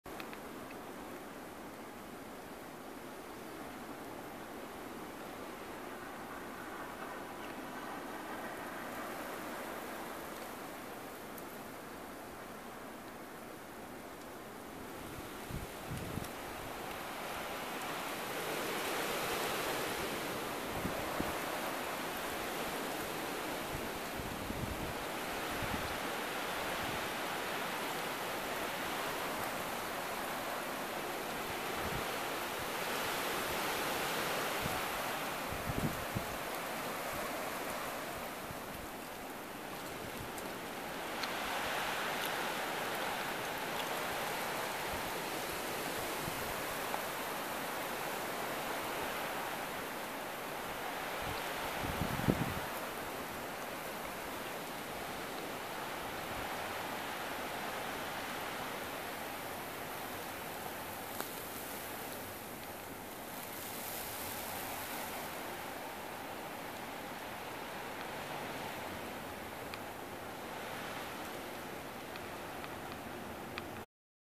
Blowing snow the day after a snow storm in Minnesota
** Set the video to full screen to see it clearer** Blowing snow the day after a snow storm in Minnesota. Beautiful snow on tree, wind, blowing snow, sound of the wind. Snow on roof, snow dune, sun in the winter. Windchill was -20 when this was filmed outside of my workplace.

This is a silent video portrait the day after a storm.